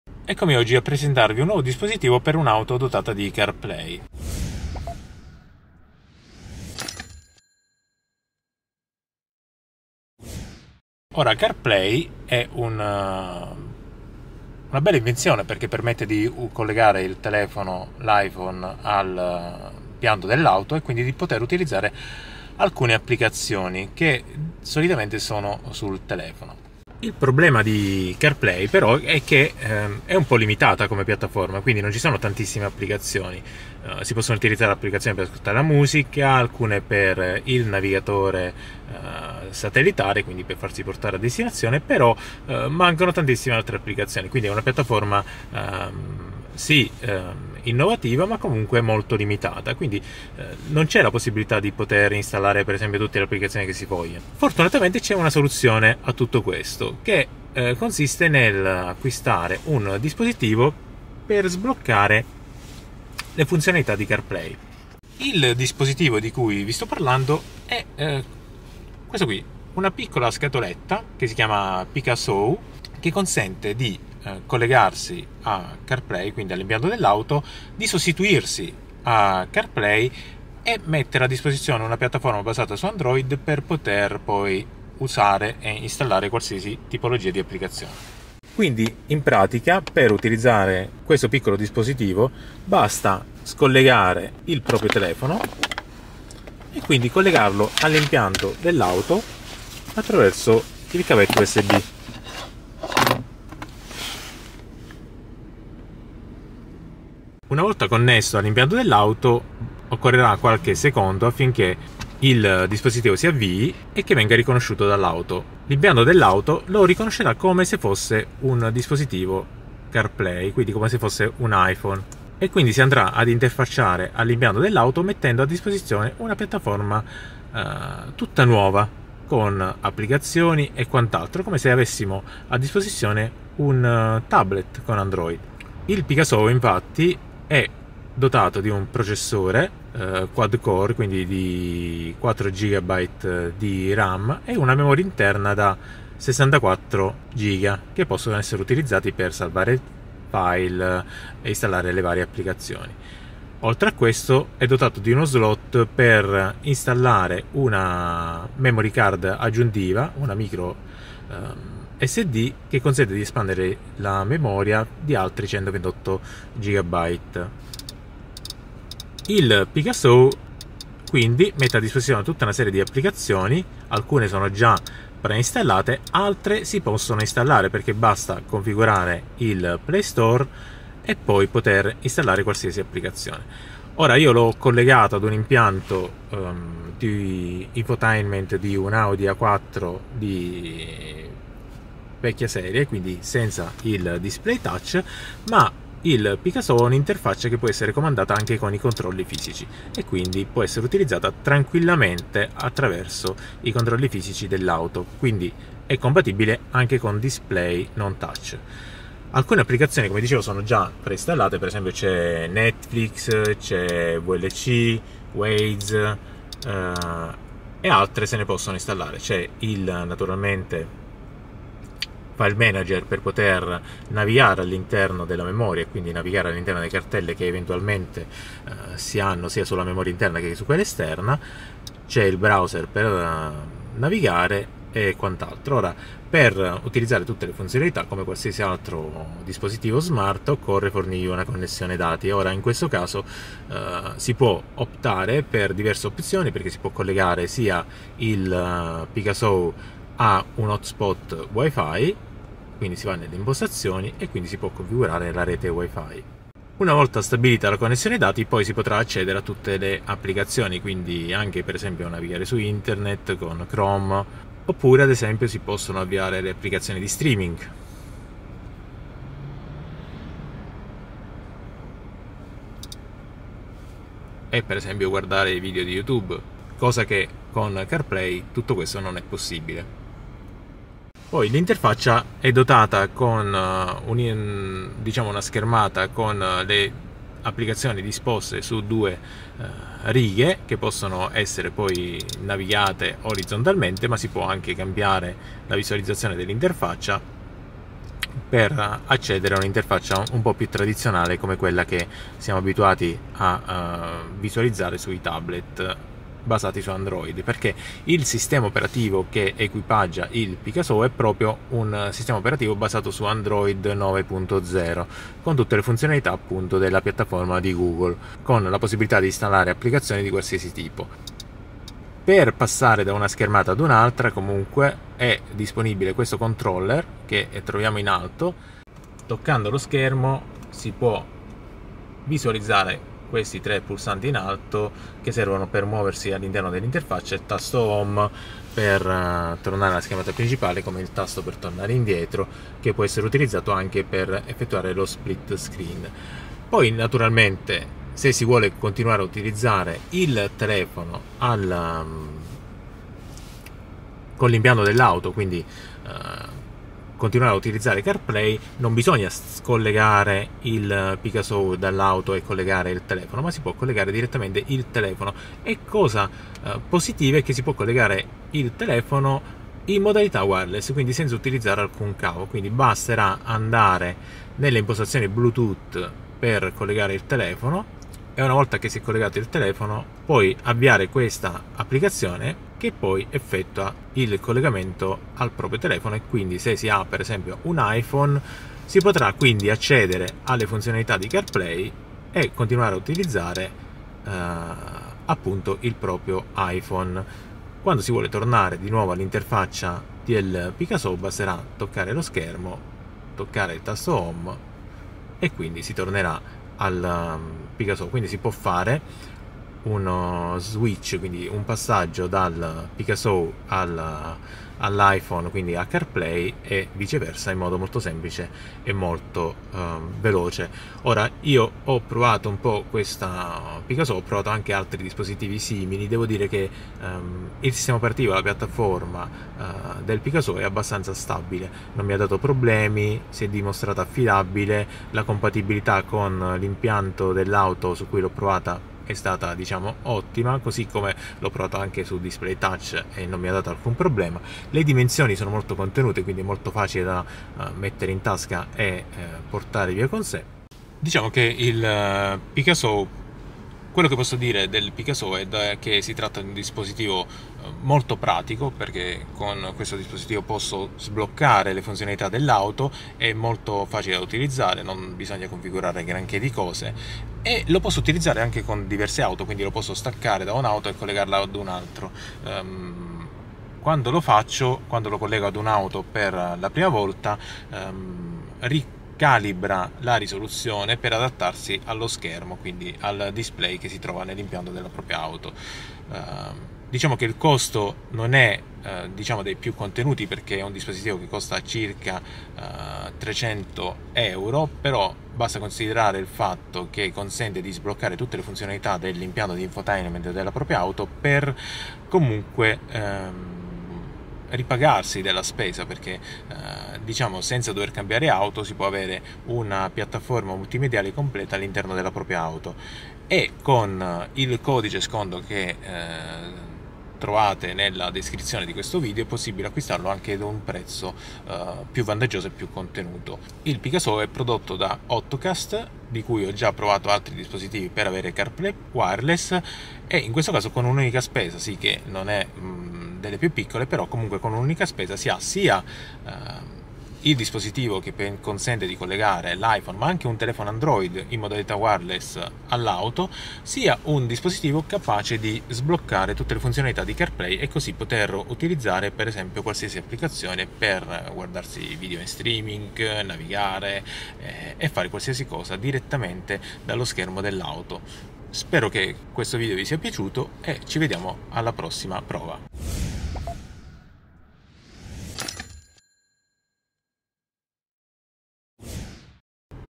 Unboxing Ottocast Picasou AI Box, il piccolo dispositivo per sbloccare CarPlay senza jailbreak
Ottocast Picasou AI Box è un piccolo dispositivo per avere CarPlay Wireless e installare qualsiasi app sull'impianto di infotainment della propria auto. Compatibile anche con Android Auto.

Negozio Ottocast Picasou Ai Box